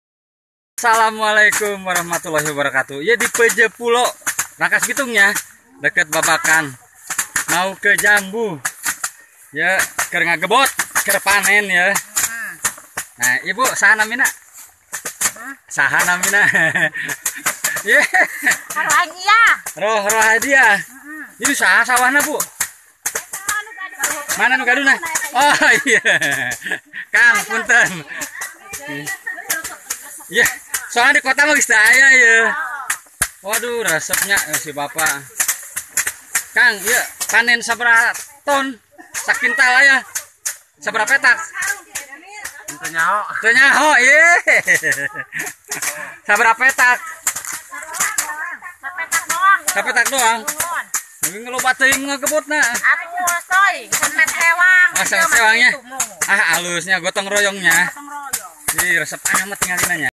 Tradisi gebot padi mulai hilang - Sketsa
Sawah-sawah yang menguning di pinggiran kota sudah hampir punah, kini berganti beras impor. Tradisi gotong royong gbot padi pun tidak lagi dikenal oleh generasi milenial; mereka tahunya tinggal makan. Saya masih sempat melihat di Rangkasbitung. (OPPO A3s)